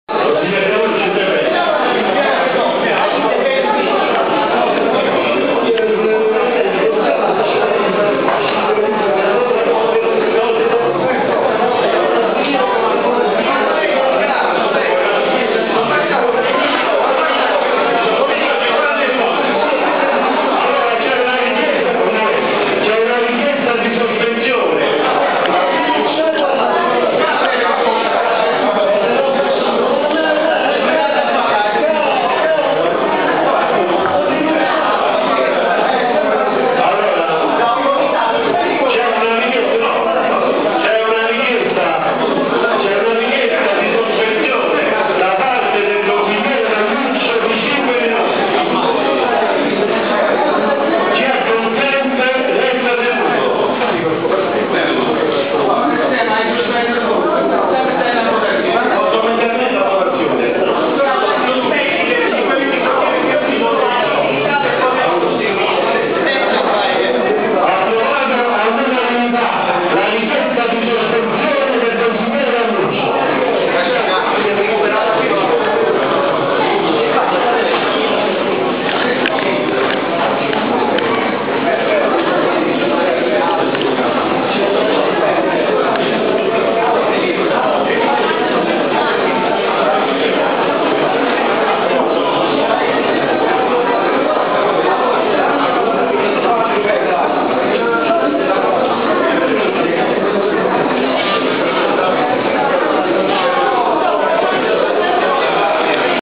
Casoria 28/02/2013 - Consiglio Comunale (08)
"Casoria Ieri & Oggi online" presenta:
Casoria giovedì 28 febbraio 2013 il Consiglio Comunale è convocato in sessione straordinaria di prima convocazione alle ore 17,30 presso la Sala Consiliare della Sede Comunale sita in Piazza Domenico Cirillo per la trattazione del seguente ordine del giorno:
Fase concitata del consiglio comunale e sospensione dei Lavori